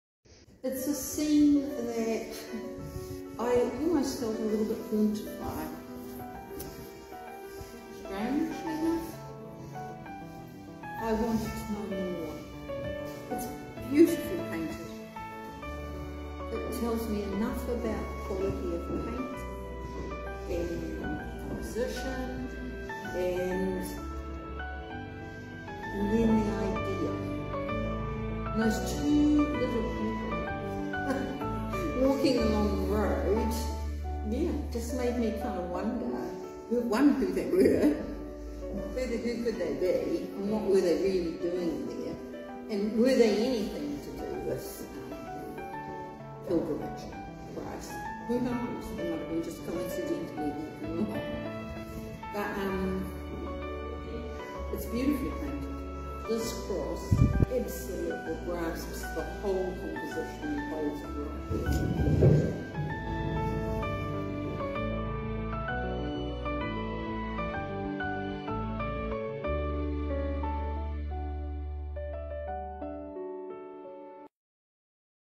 A European Christian Pilgrim
David John Clark won the 2023 art award "Age is a Work of Art" for his painting "A European Christian Pilgrim Pathway". Judge Gaye Jurisich, discusses why she chose this artwork for the 2023 art awards.